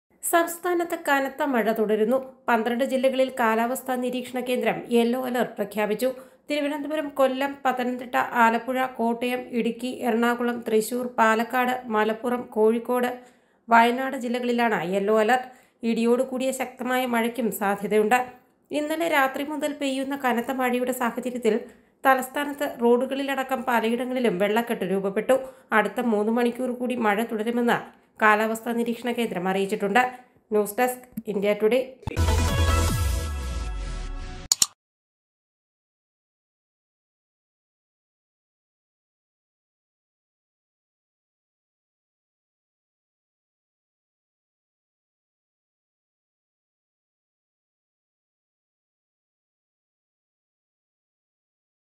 സംസ്ഥാനത്ത് കനത്ത മഴ: 12 ജില്ലകളിൽ യെല്ലോ അലർട്ട് Heavy Rain in Kerala Yellow Alert in 12 districts
keralaweather indiatodaymalayalam keralarain | kerala rain | kerala weather | India Today Malayalam
ഇന്നലെ രാത്രി മുതൽ പെയ്യുന്ന കനത്ത മഴയുടെ സാഹചര്യത്തിൽ തലസ്ഥാനത്ത് റോഡുകളിൽ അടക്കം പലയിടങ്ങളിലും വെള്ളക്കെട്ട് രൂപപ്പെട്ടു.